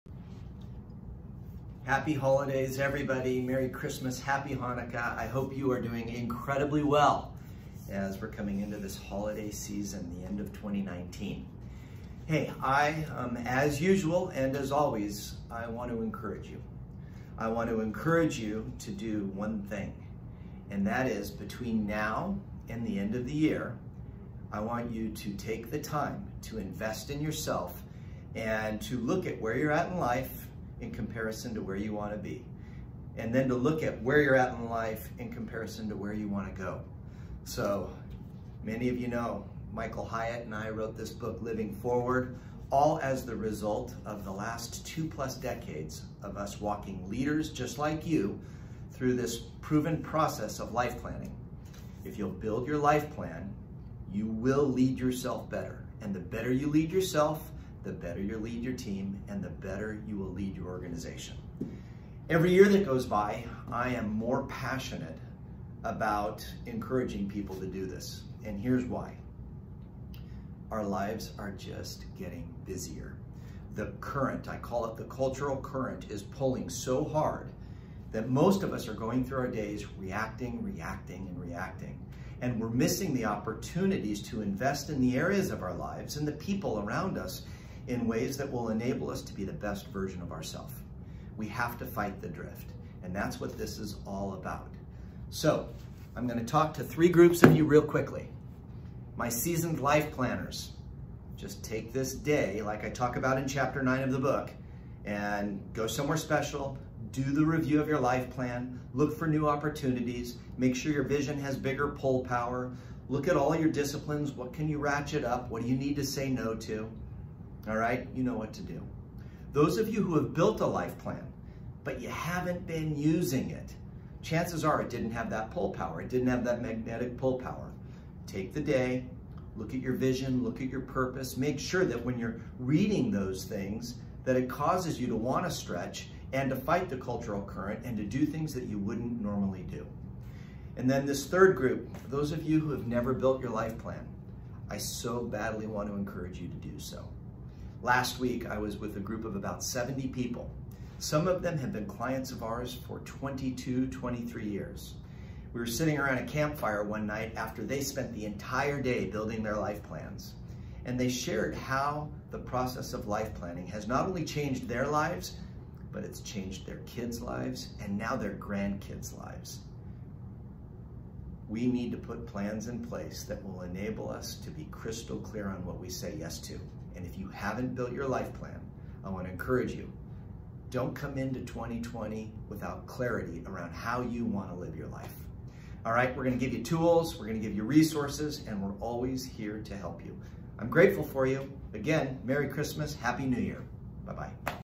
Living Forward with Daniel Harkavy
As 2019 winds down, we want to encourage you to either revisit your life plan, or write your life plan. In this video, Building Champions Founder & CEO, Daniel Harkavy, walks you through why now is the perfect time to reflect on where you have been and put plans in place for where you want to go, and how you'll get there. We hope you enjoy the process as much as the outcome!